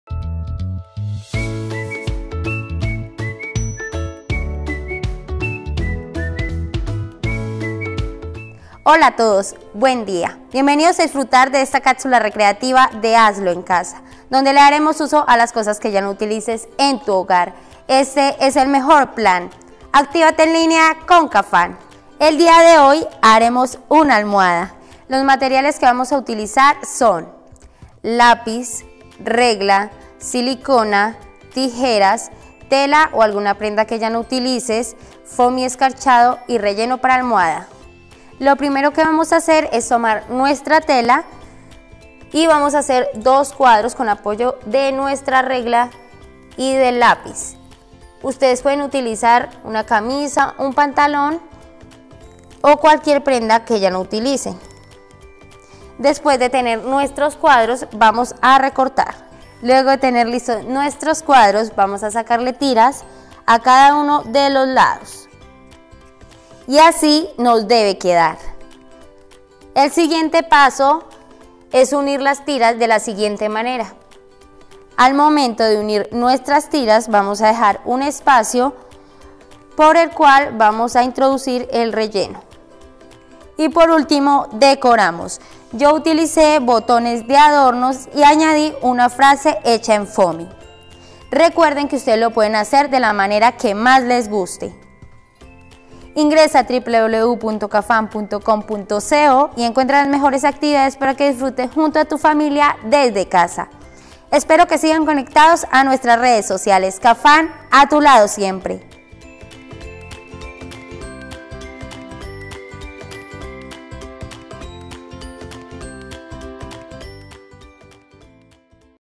TallerCreativo - Almohada
¿Alguna vez has construido tu propia almohada? Pues ha llegado el día, consigue los materiales y vamos a trabajar con Cafam.